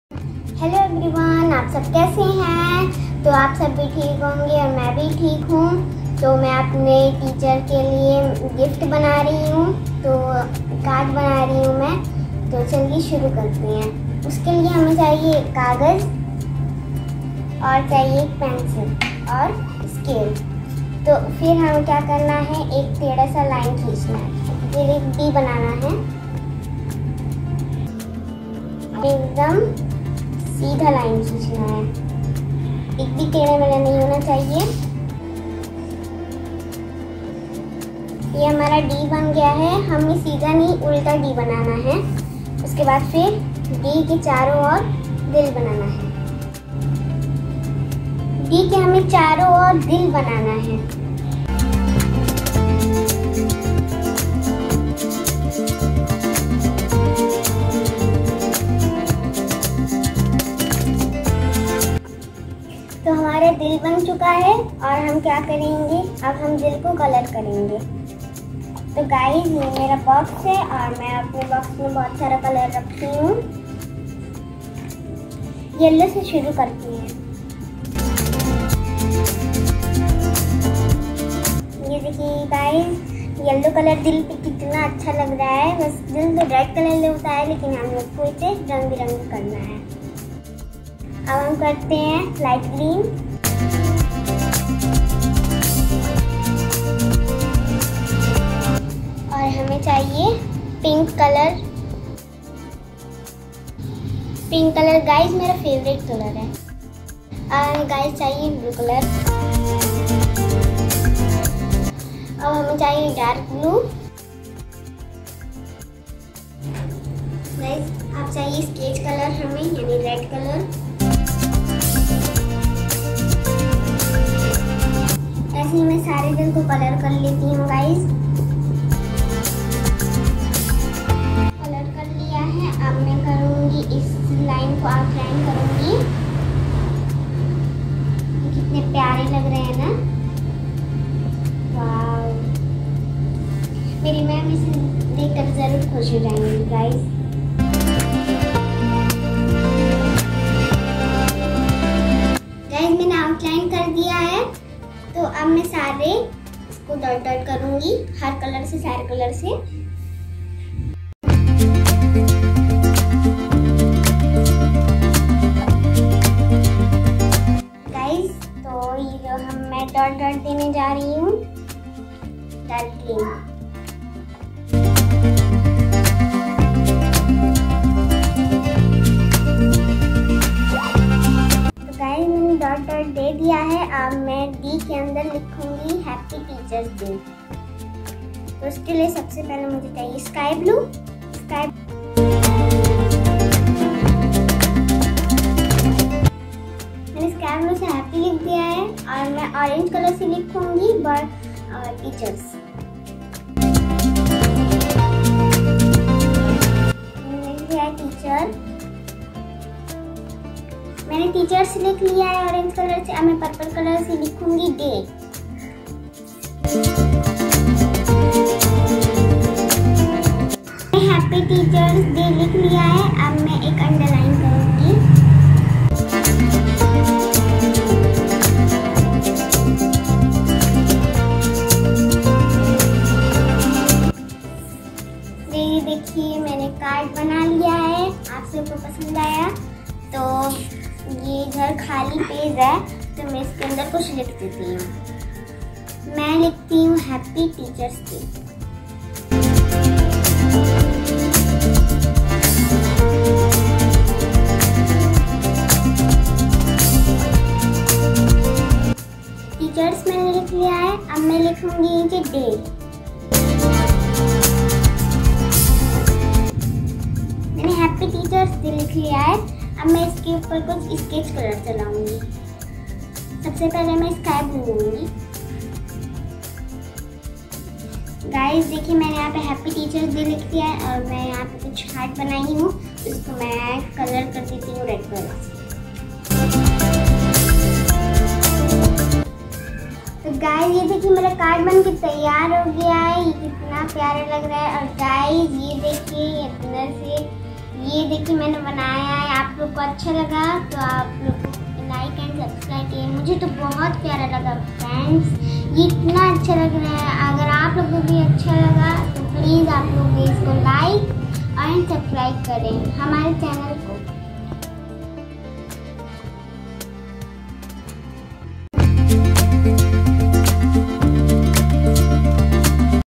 Happy Teachers Day Special Card Making a Cute Girl Hand Made Card
happy teachers day special card
Hand Made